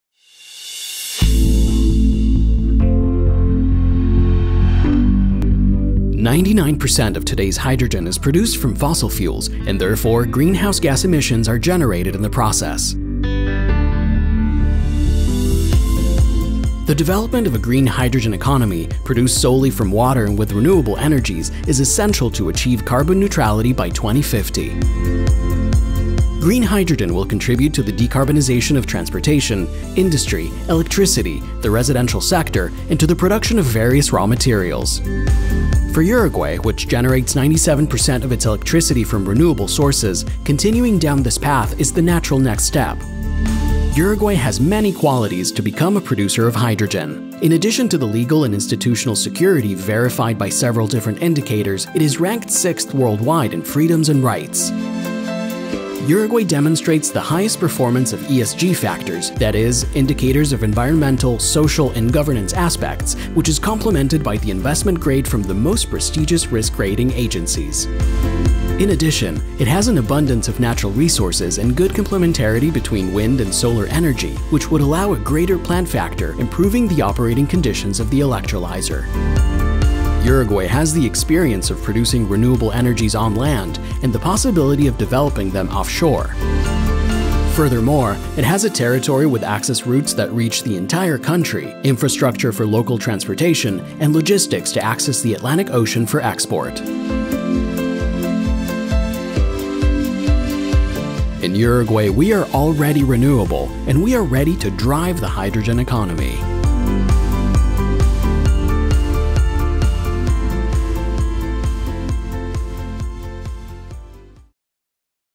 Green Hydrogen, a key link to complete the second energy transition in Uruguay.
The objective of this initiative is to develop the first Green Hydrogen pilot project in Uruguay, from the articulation of the public and private sector efforts.